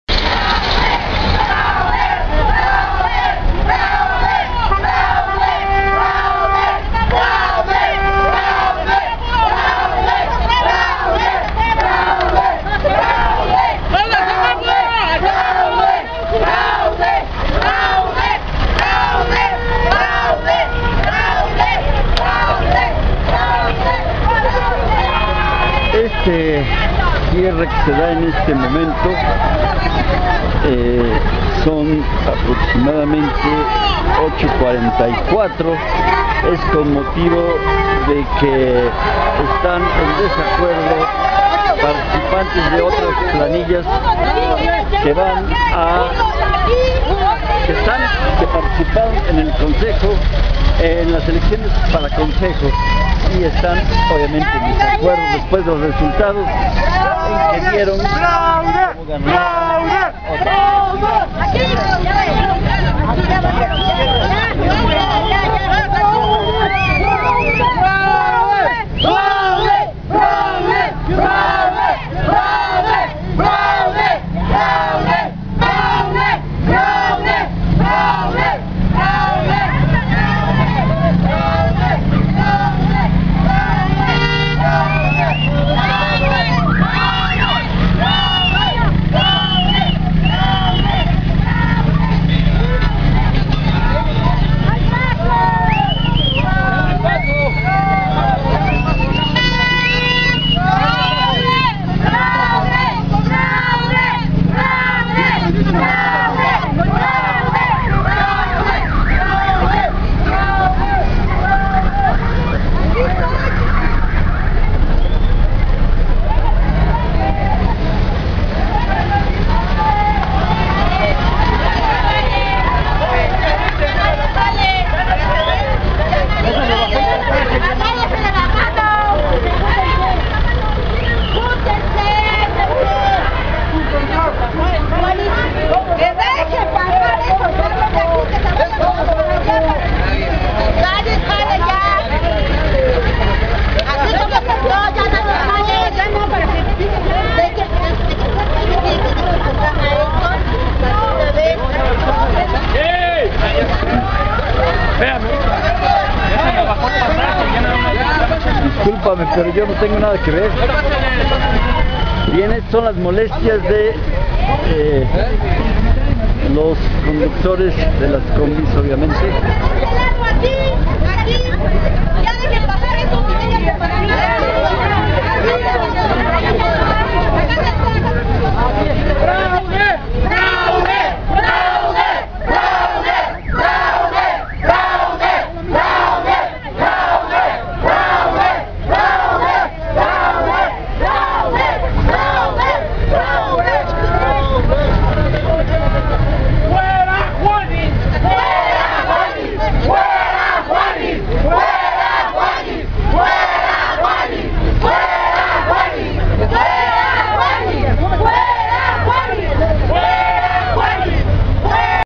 Bloquean San Cristóbal Centro inconformes por votación; acusan fraude de Planilla 1 en Ecatepec
aquíEcatepec.- Vecinos inconformes con los resultados de la elección para Consejo de Participación Ciudadana de San Cristóbal Centro, en el municipio de Ecatepec, bloquearon el cruce de Avenida Morelos y calle Juárez cerca de las 21:00 horas. A las 21:35 se presentaron autoridades de la oficina de la Casa de Gobierno del Estado de México en este municipio, y ambas partes trasladaron al lugar.

Todo el grueso del contingente, aunque aquí ya no se aprecia, se fue a negociar a dicho inmueble ubicado en la Calle del Sol, atrás del Mercado Municipal, y así quedó liberada la vialidad... y su presunta movilización.